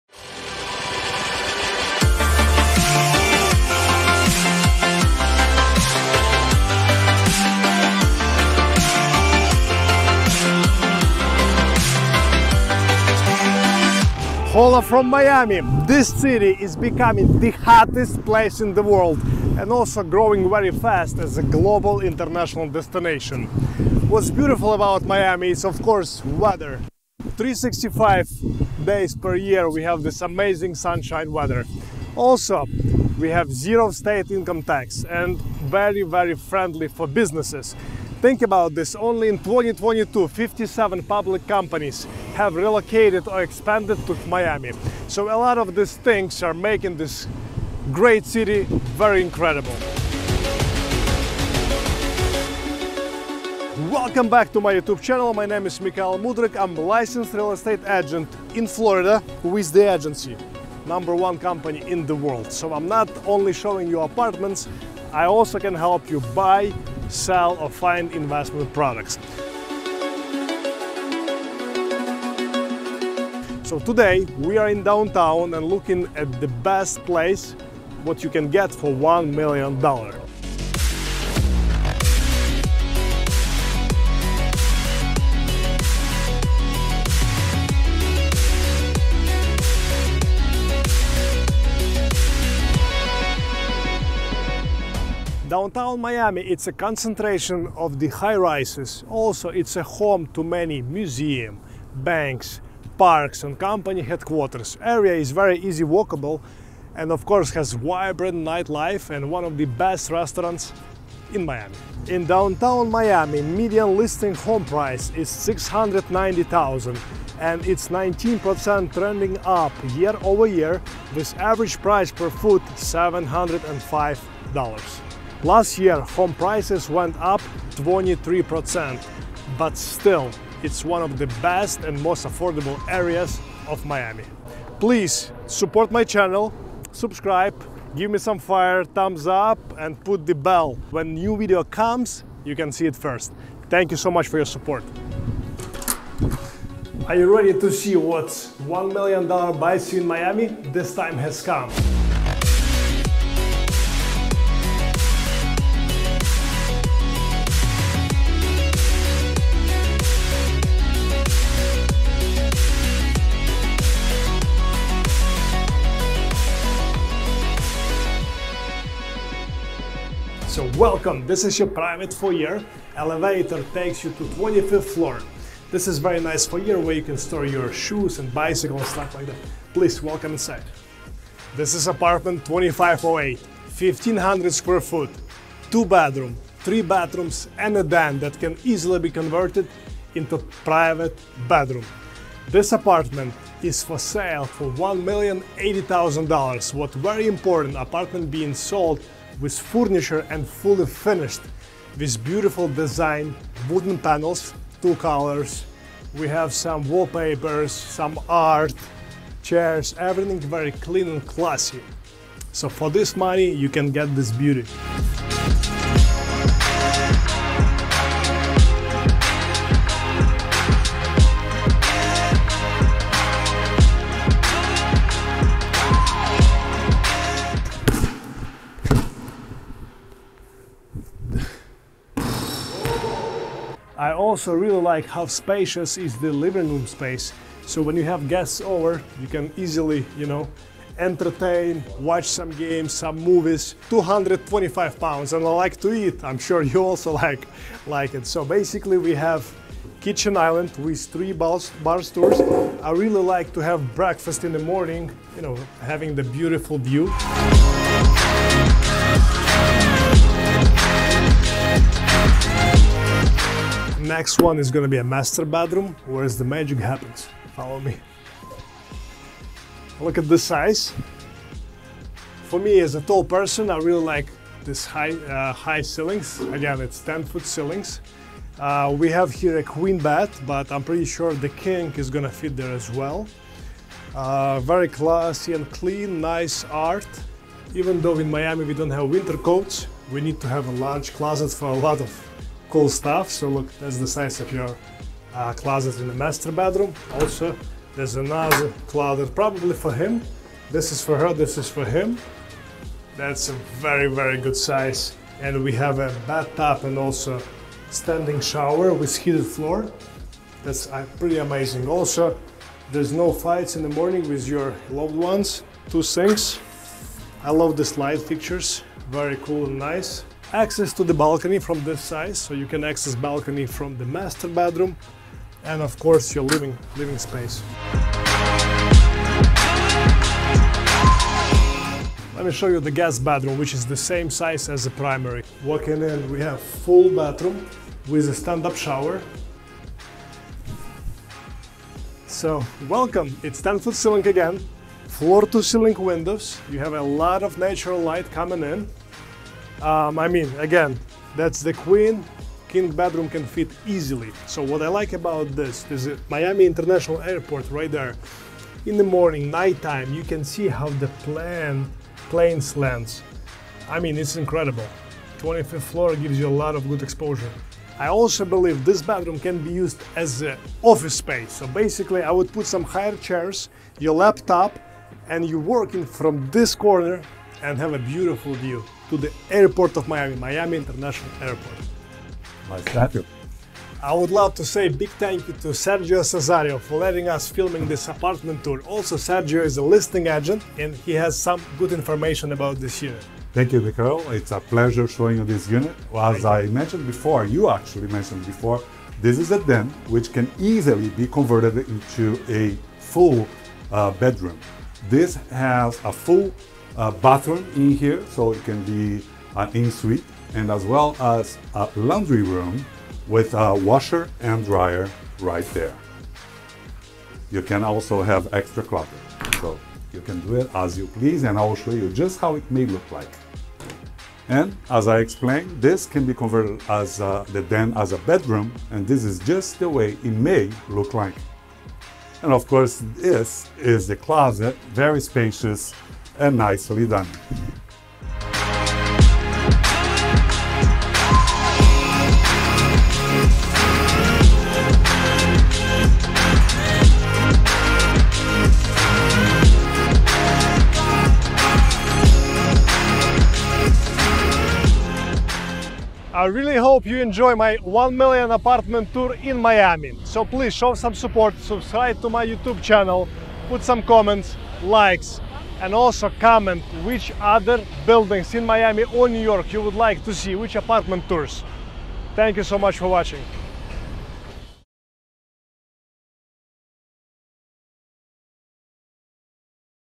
What $1,000,000 buys you in Miami? | Luxury Apartment Tour | Mikhail Mudrik
What $1,000,000 buys you in Miami? | Miami Luxury Real Estate | Mikhail Mudrik

In this video, we’re going to visit Paramount Miami Worldcenter luxury apartment on 25th floor.

What’s good about Miami and why you should consider it as a good option for real estate investment? What you can get for $1,000,000? Which perks this Paramount Miami Worldcenter luxury apartment has?

Watch till the end, this is going to be helpful!
00:00 - What’s good about Miami?
00:53 - About me
01:21 - Downtown Miami
02:25 - What 1M$ buys you in Miami?
02:50 - 25-floor apartment tour!
04:24 - Magical Bedrooms
05:55 - Watching planes out of the window